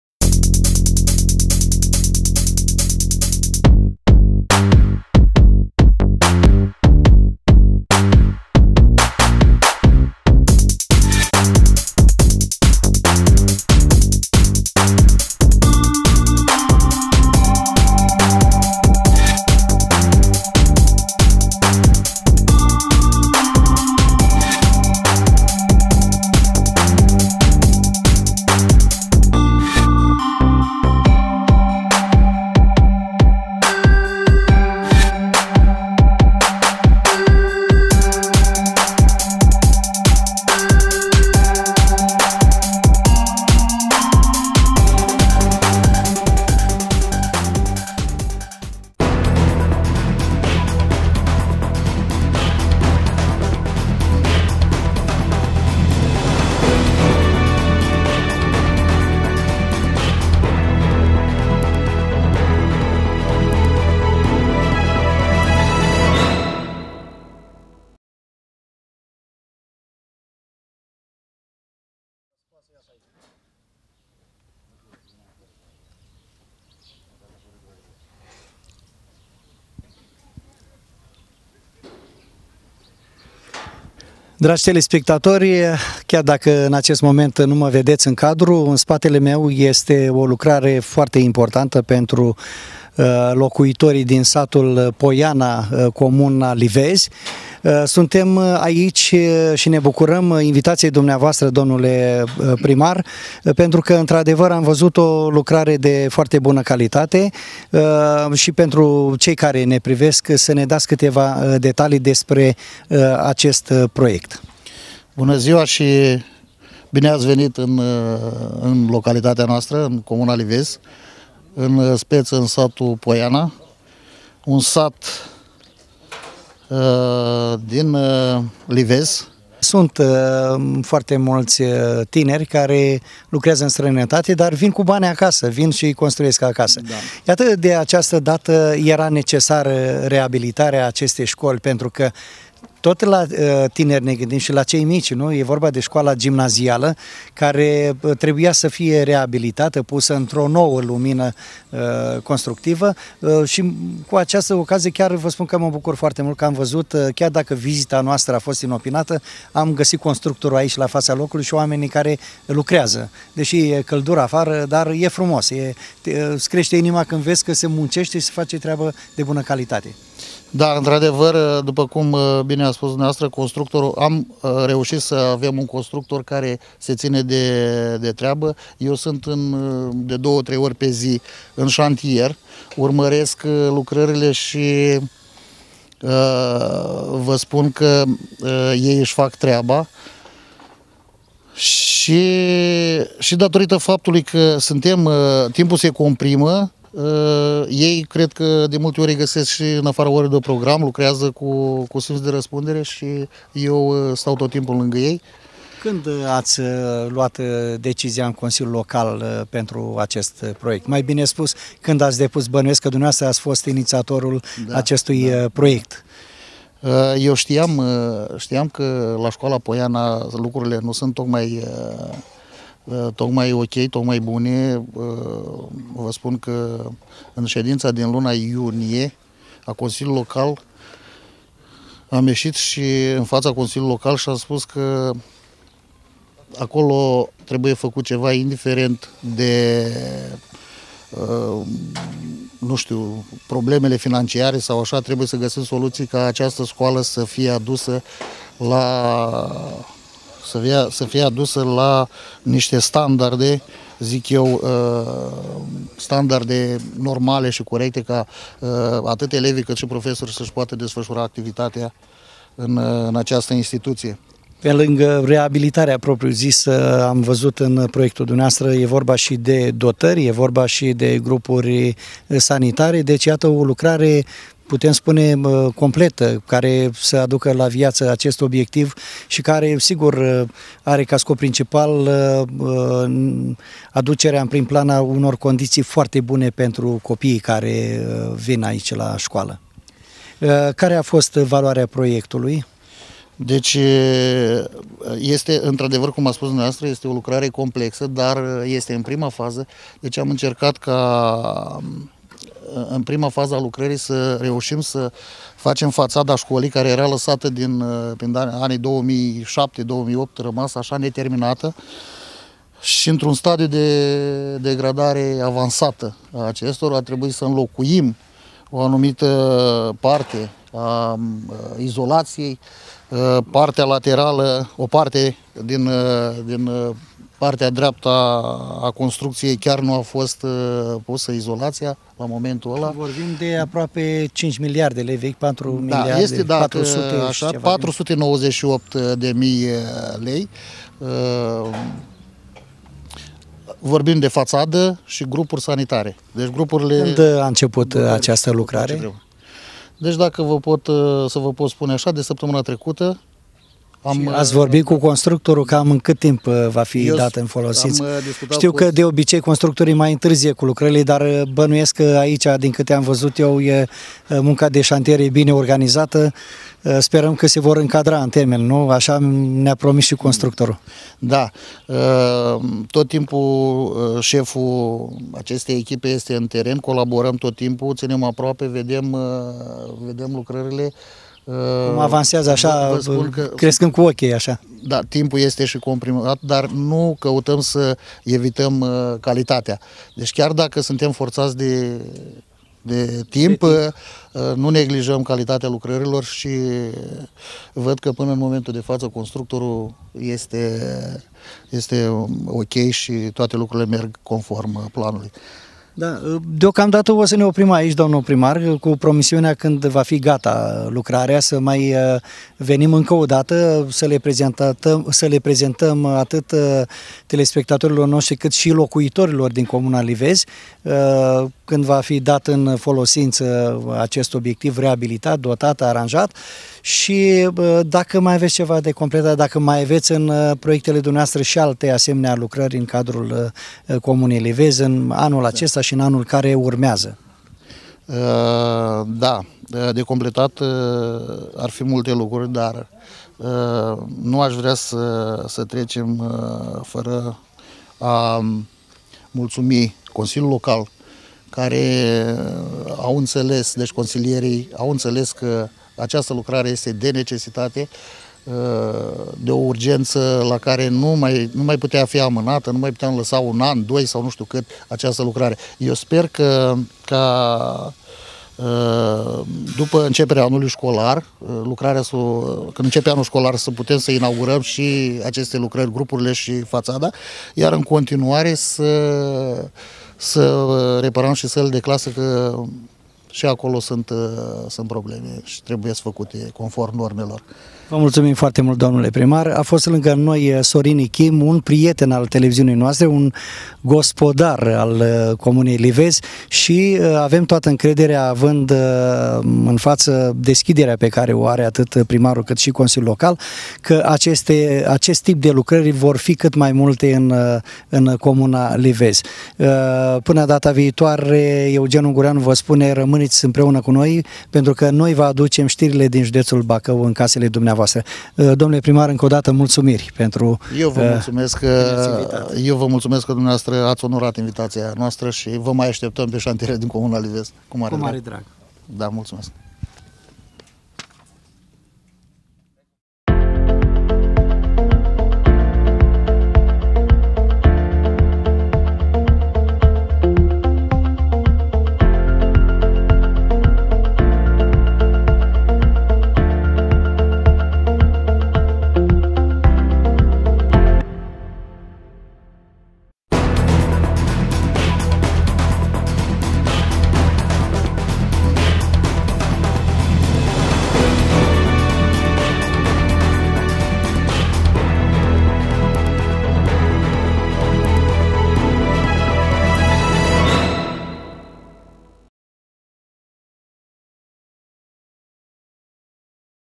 LA POIANA Interviu cu primarul Sorin Ichim FILMARE EXCLUSIV TV UHD 4K
LA POIANA, comuna Livezi, județul Bacău, Interviu cu primarul Sorin Ichim FILMARE EXCLUSIV TV UHD 4K.

Cu stimă,
echipa Exclusiv

Vizionare plăcută !